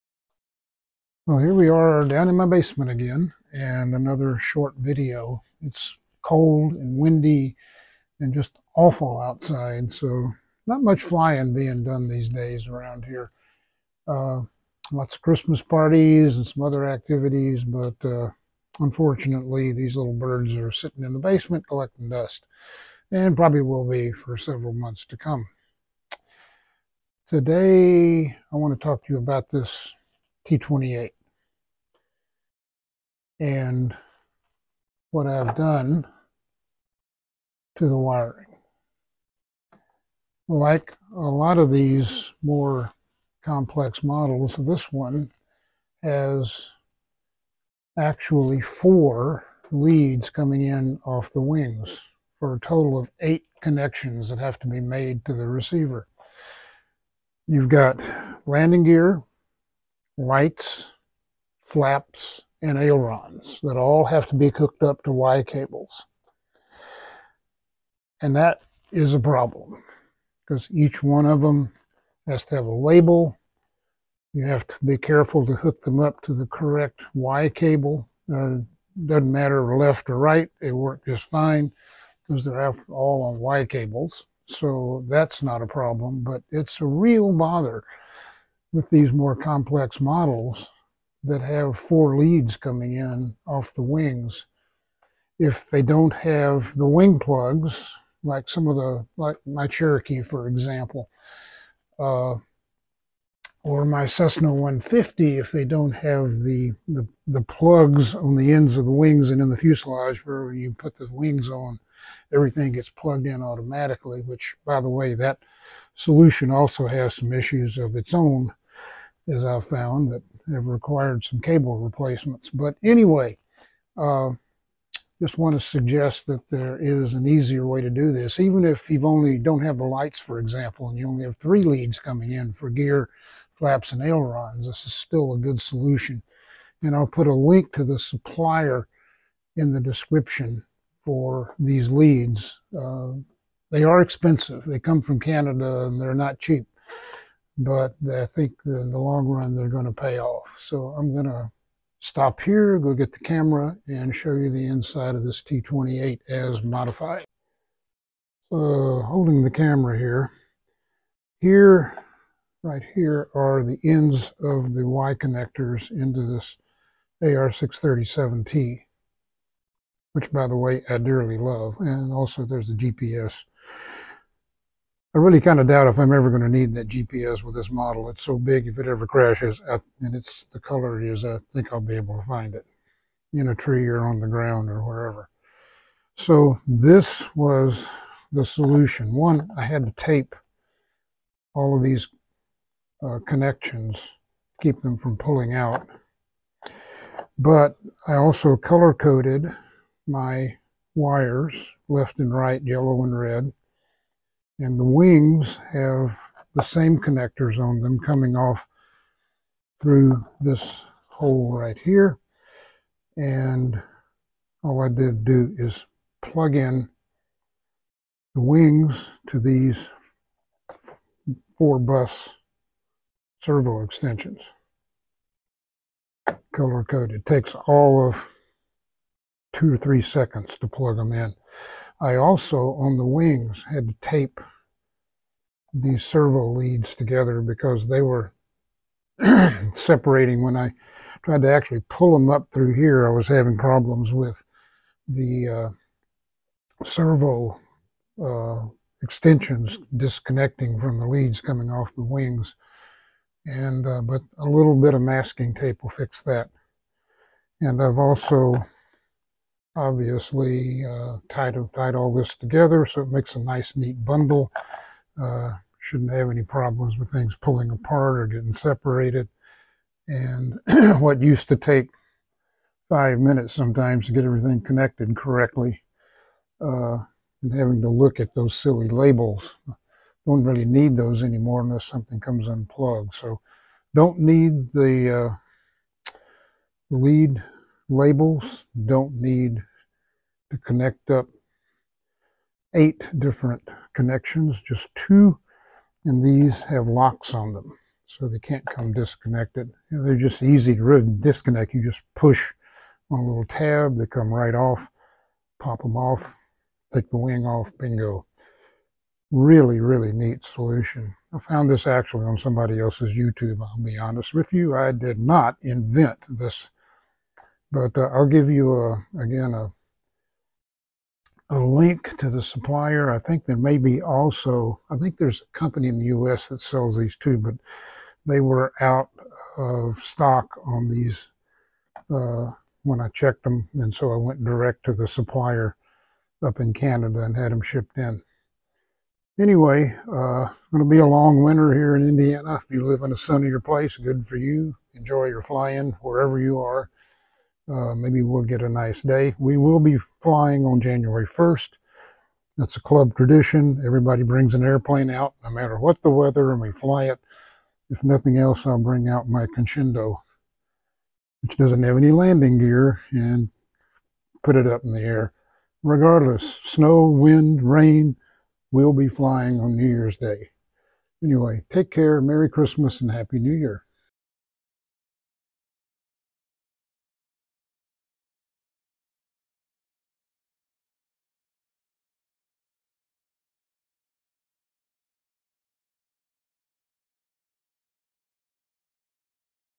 A solution for plugging in multiple wing servos to receiver y-cables
My 2-meter T-28 and other similar models have multiple wing servo leads to be plugged in every time the wings are installed. This model has to be taken apart for transport, so it is a bother. The T-28 has 8 wing leads, and they are labelled at each end. Matching them up and plugging them in takes time and is error prone. I installed extensions that allow all 8 to be consolidated into only 2 color-coded plugs.

I found 2 suppliers. The Canadian manufacturer site's link to this item is:

(I receive no compensation for these recommendations.)

You will need to get 2, one for each wing. The video shows the result for the fuselage, the wings have the male extensions that plug into the fuselage via the fuselage hole shown.

They also carry a similar product for 3 leads per wing.